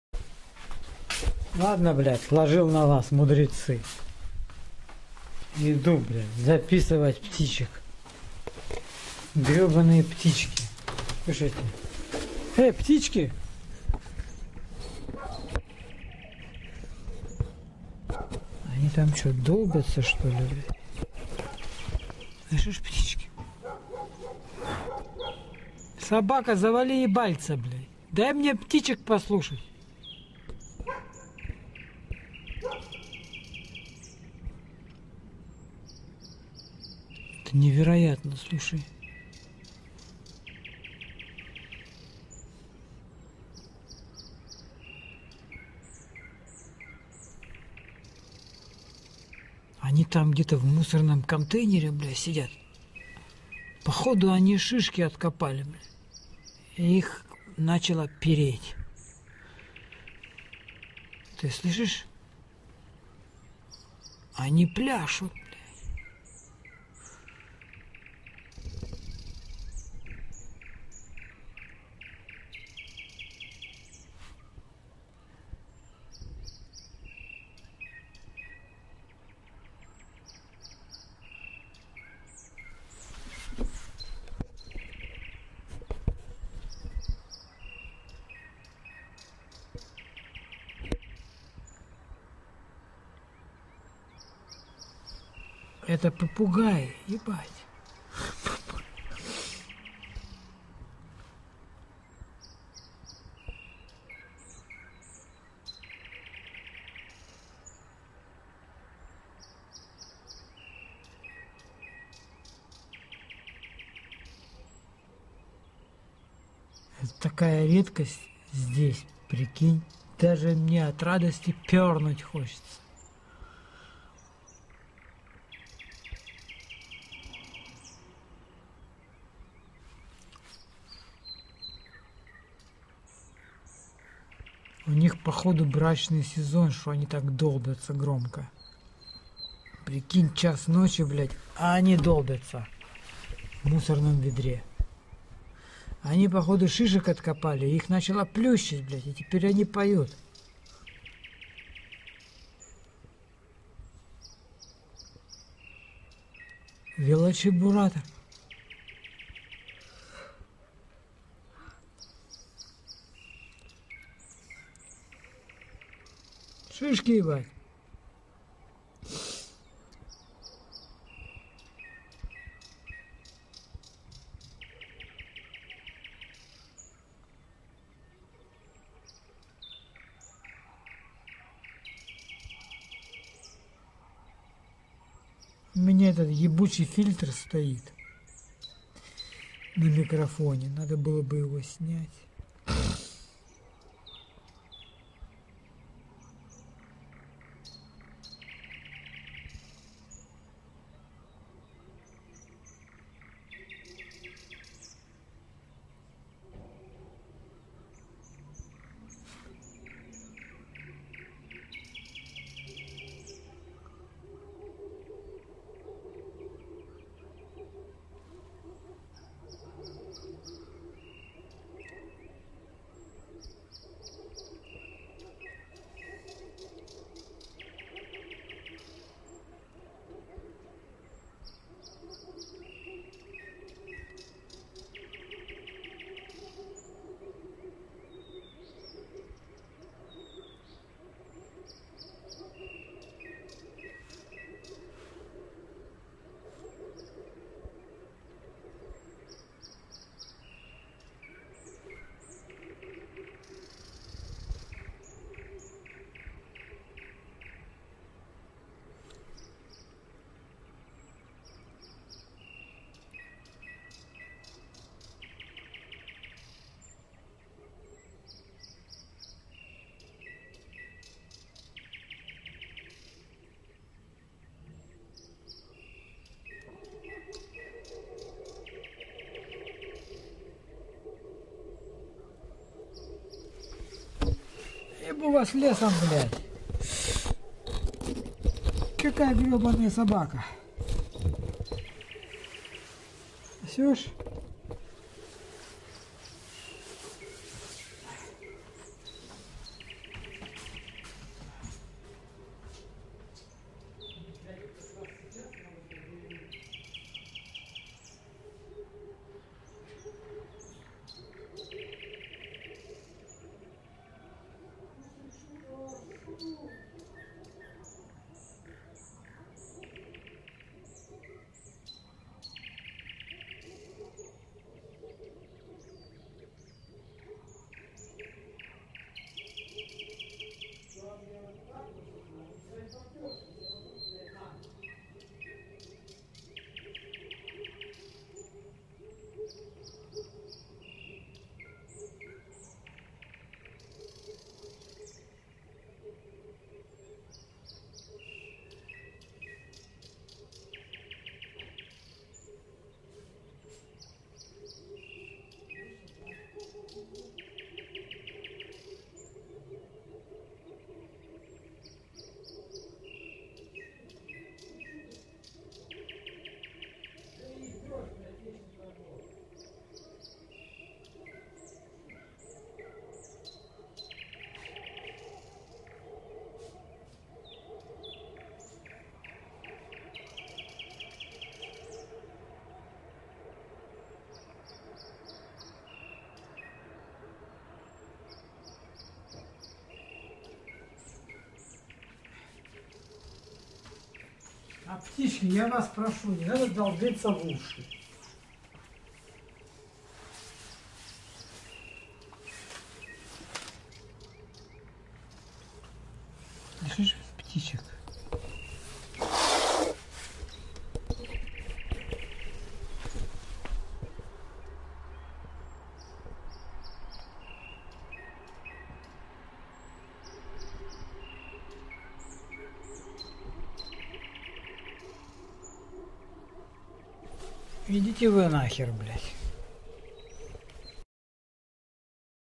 слушай как птички поют в ботаническом саду LIVE микрофон
listen the birds are singing in the Botanical garden  велочебуратор LIVE  sound   = и , чирикалы в ночи  долбят клювом кирпичи